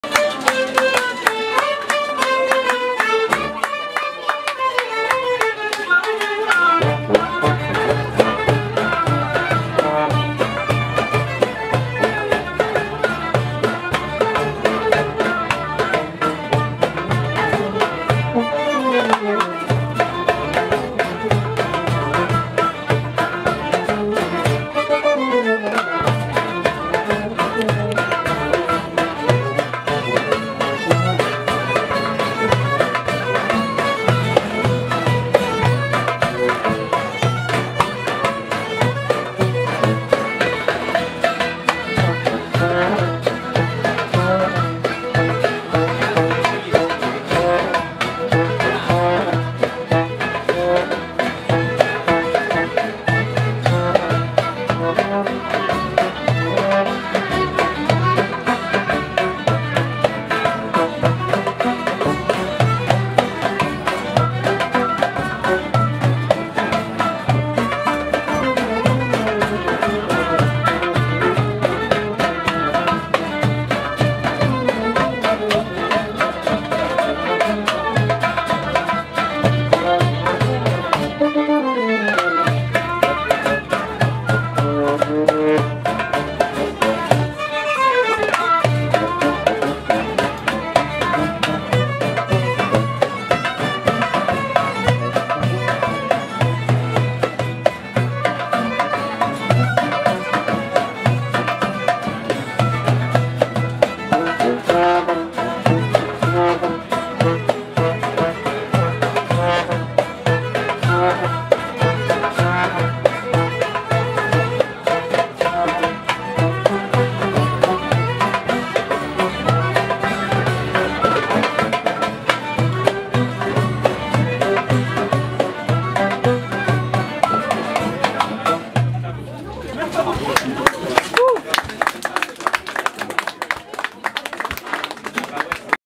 03/05/15 Happy Birthday. Lieu d’Europe à Strasbourg a 1 an. Musiciens Papyros’N. Videos 3/7 POUSSIN
Happy Birthday Lieu d’Europe : Un an déjà que le Lieu d’Europe vous propose de mieux comprendre l’histoire de la construction européenne et de participer à la réflexion sur l’avenir de l’Europe. Venez souffler cette 1ère bougie en compagnie des musiciens Papyros’N.                                                                                                       Lieu : le Lieu d’Europe : 8 rue Boecklin, Strasbourg
Organisateur : Conseil de l’Europe en partenariat avec le Centre d’information sur les Institutions Européennes.